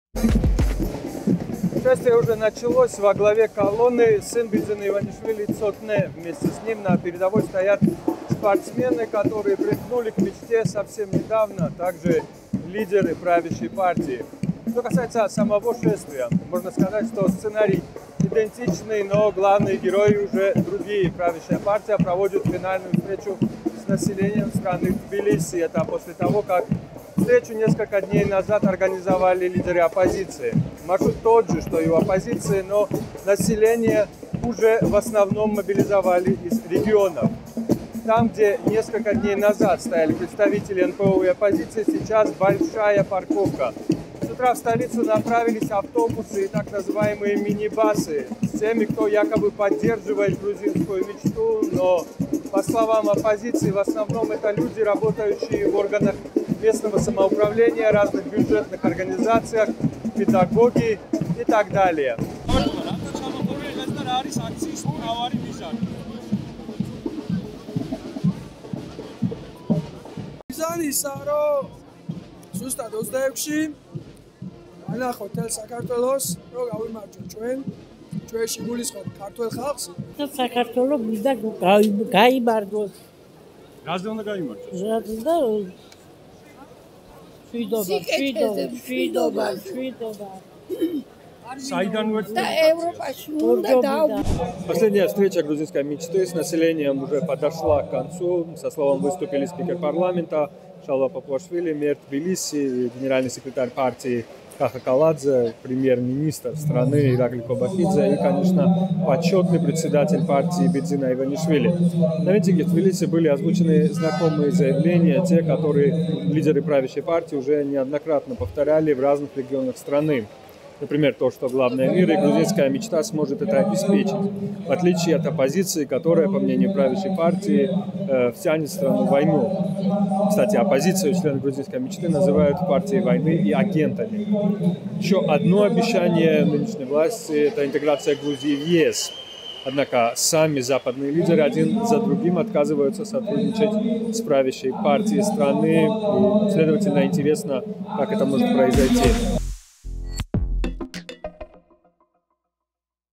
Предвыборный митинг «Грузинской мечты»
🚌 Сторонников «Грузинской мечты» и госслужащих свезли на митинг в Тбилиси со всей страны.